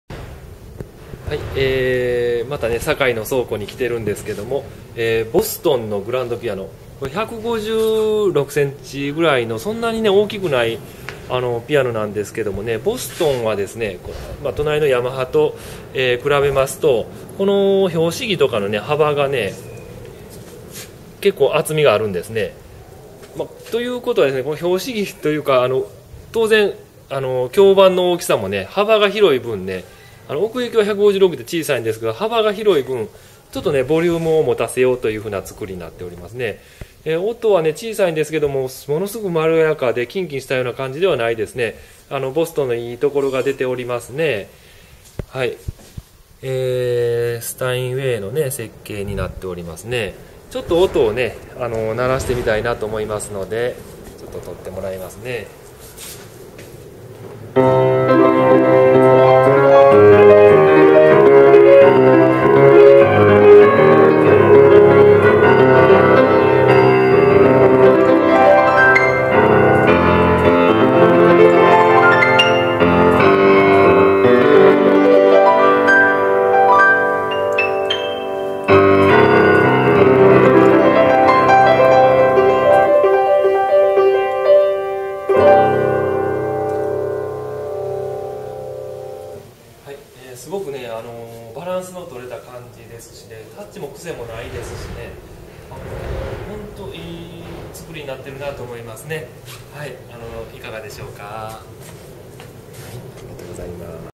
ボストン156Ⅱ程度の良いのがありました。（中古ピアノ）
100までピアノライフです。ピアノ仕入れ先の倉庫でこれは良いっと思ったので撮影しました。
大阪堺市の倉庫です。興味がおありの方は試弾出来ます。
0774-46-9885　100までピアノライフ